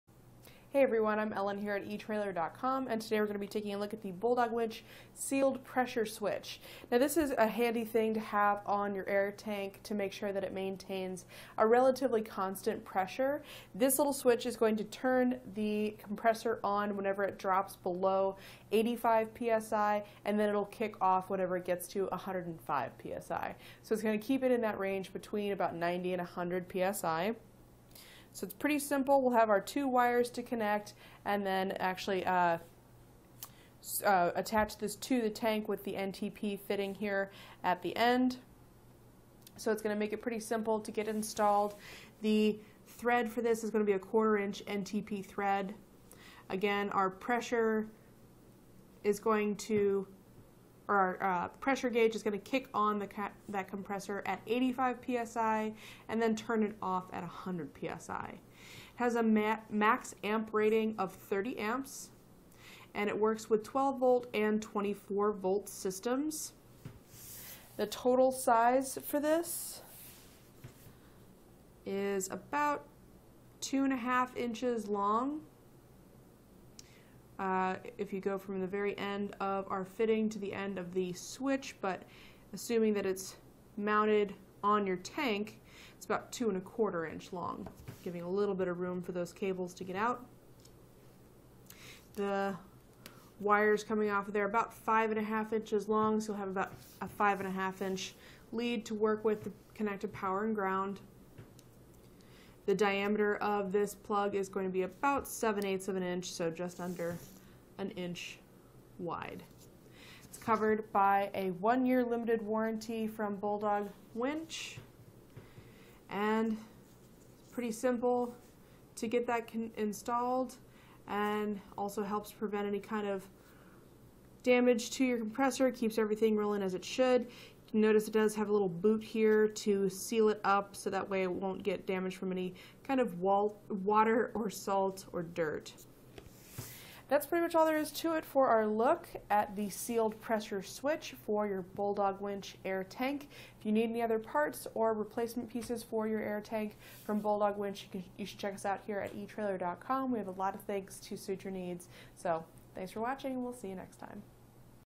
etrailer | Review of Bulldog Winch Air Compressor Parts - Sealed Pressure Switch - BDW69FR
Click for more info and reviews of this Bulldog Winch Air Suspension Compressor Kit,Vehicle Suspension:

Ellen:                                                 Hey everyone. I'm Ellen here at etrailer.com and today we're going to be taking a look at the Bulldog Winch Sealed Pressure Switch. Now this is a handy thing to have on your air tank to make sure that it maintains a relatively constant pressure. This little switch is going to turn the compressor on whenever it drops below 85 PSI and then it'll kick off whenever it gets to 105 PSI, so it's going to keep it in that range between about 90 and a hundred PSI, so it's pretty simple. We'll have our two wires to connect and then actually attach this to the tank with the NTP fitting here at the end. So it's going to make it pretty simple to get installed.The thread for this is going to be a quarter inch NTP thread.

Again, our pressure is going to. Or our pressure gauge is going to kick on that compressor at 85 PSI and then turn it off at a hundred PSI. Has a max amp rating of 30 amps and it works with 12 volt and 24 volt systems. The total size for this is about two and a half inches long. If you go from the very end of our fitting to the end of the switch, but assuming that it's mounted on your tank, it's about two and a quarter inch long, giving you a little bit of room for those cables to get out.

The wires coming off of there about five and a half inches long, so I have about a five and a half inch lead to work with the connected power and ground.The diameter of this plug is going to be about seven eighths of an inch, so just under an inch wide. It's covered by a one year limited warranty from Bulldog Winch and pretty simple to get that installed and also helps prevent any kind of damage to your compressor. It keeps everything rolling as it should. If you notice, it does have a little boot here to seal it up so that way it won't get damaged from any kind of water or salt or dirt.That's pretty much all there is to it for our look at the Sealed Pressure Switch for your Bulldog Winch air tank. If you need any other parts or replacement pieces for your air tank from Bulldog Winch, you should check us out here at etrailer.com.